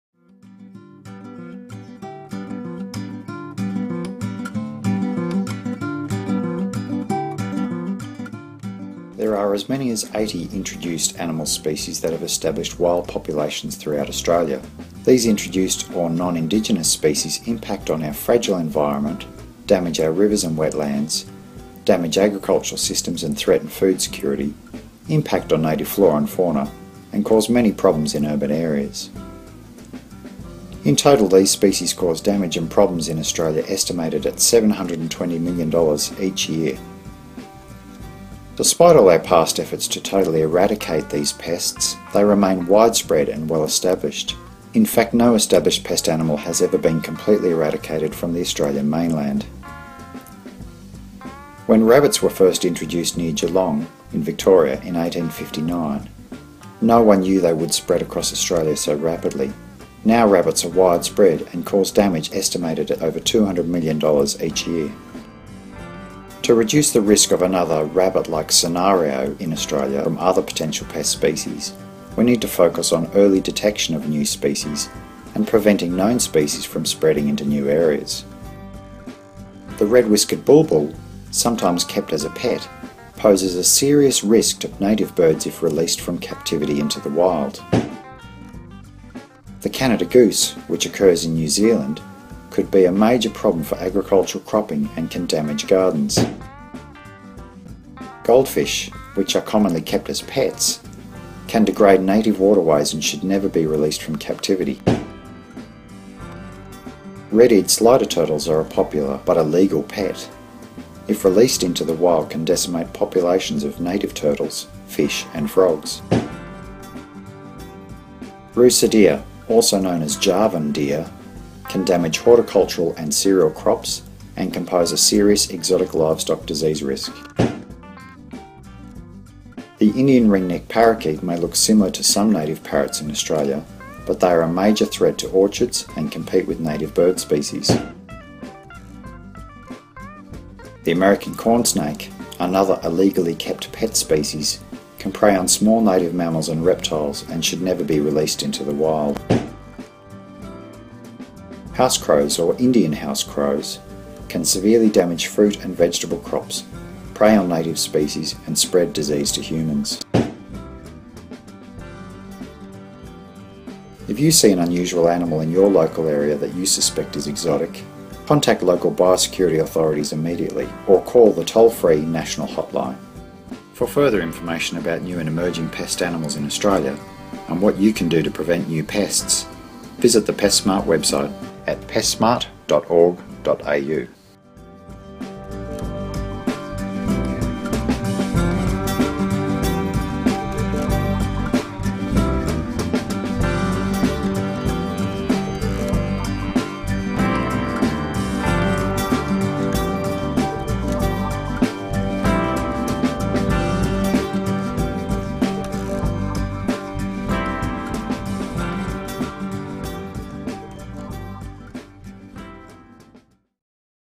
New and Emerging Pest Animals
There are as many as 80 introduced animal species that have established wild populations throughout Australia. These invasive species impact our fragile environment, damage our rivers and wetlands, damage agricultural systems and threaten food security, impact on native flora and fauna, and cause many problems in urban areas. No established pest animal has ever been completely eradicated from the Australian mainland.

To reduce the risk of new exotic invasive pests becoming established in Australia, we need to focus on early detection of new species, and preventing known species from spreading into new areas.

This video takes a look at some of the unwanted invasive species that pose risks to Australia should they establish wild populations.
Sightings of unusual animals in the wrong place should be reported to biosecurity authorities.

This project was funded under the Australian Pest Animal Research Program (APARP).